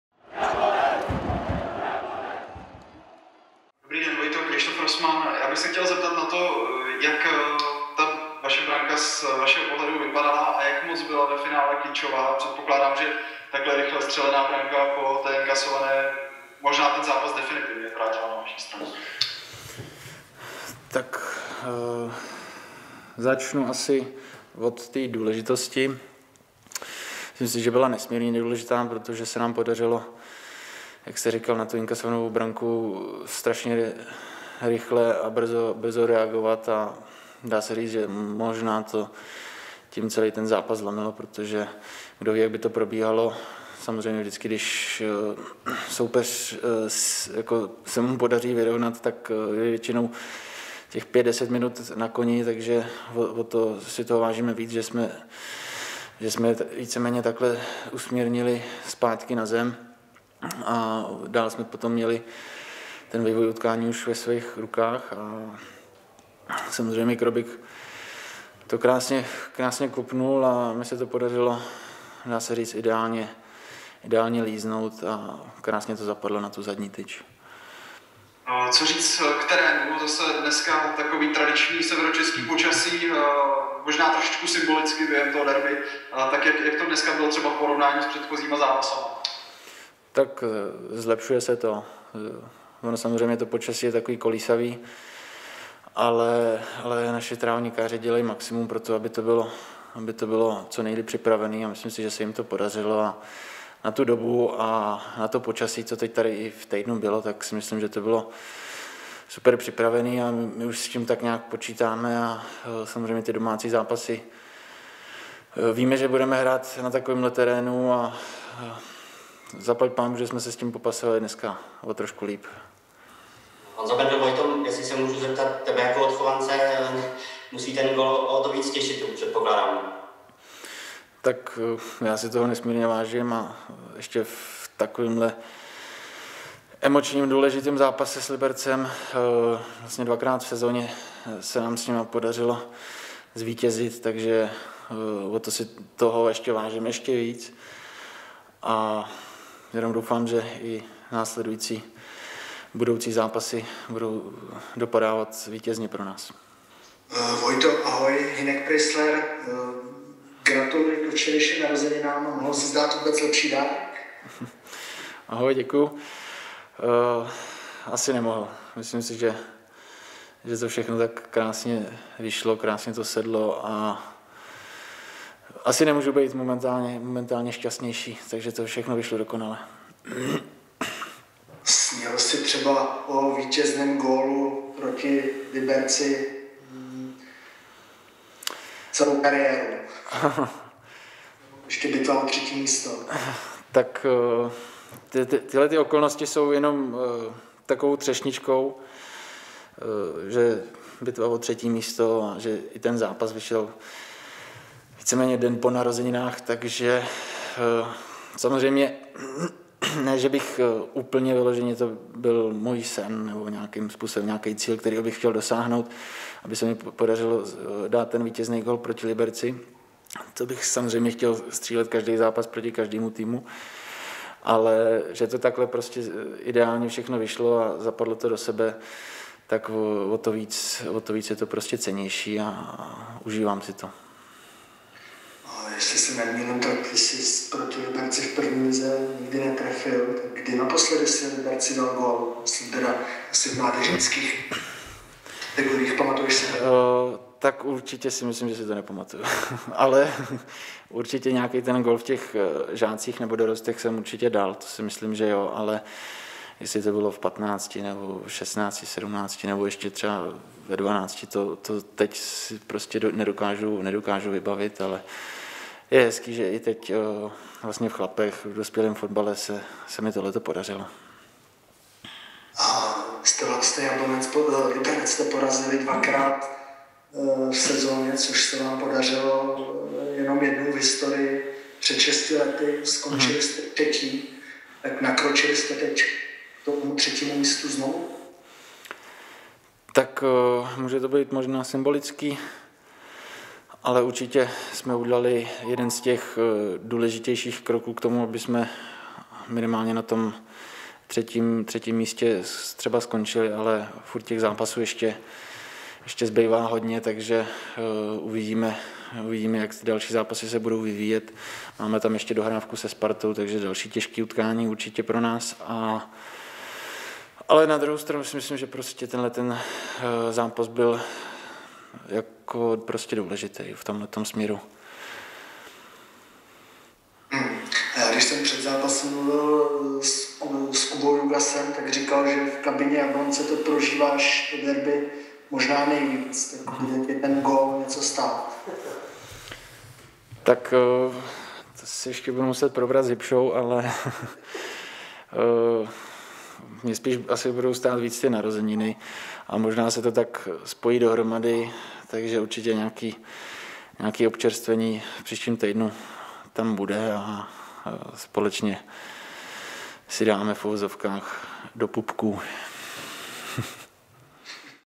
Střelec vítězné branky Vojtěch Kubista
V derby proti Liberci pokaždé cítí Vojtěch Kubista velkou rivalitu. Jako odchovanec Jablonce se včera dočkal prvního ligového gólu proti Liberci a jeho vítězná trefa znamenala zisk důležitého vítězství.